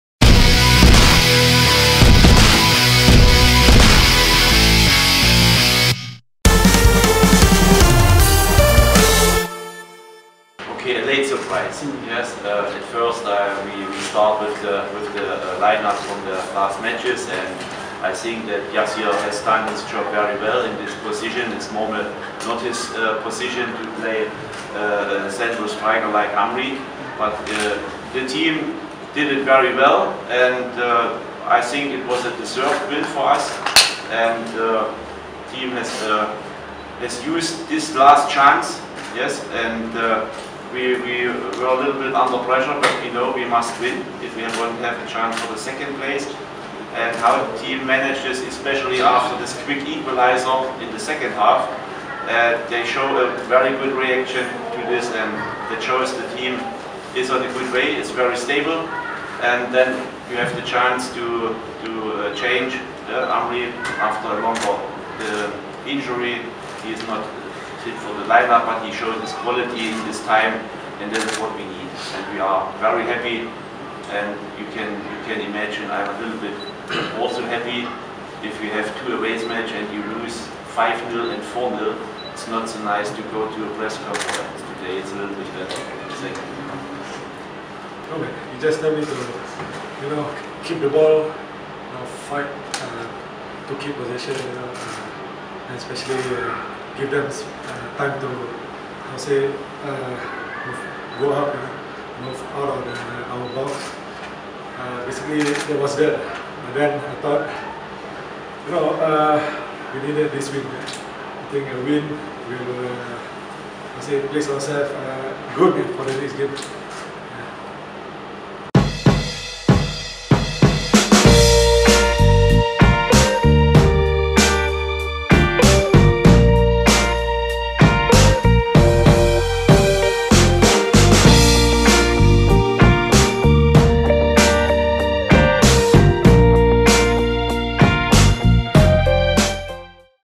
AFC Cup 2017 Press Conference (Post-Match): Felda United v. Tampines Rovers
We were represented by head coach Juergen Raab, and man of the match Khairul Amri, in the post-match press conference after our 3-1 victory last night over Felda United in the AFC Cup on 19 April in Malaysia.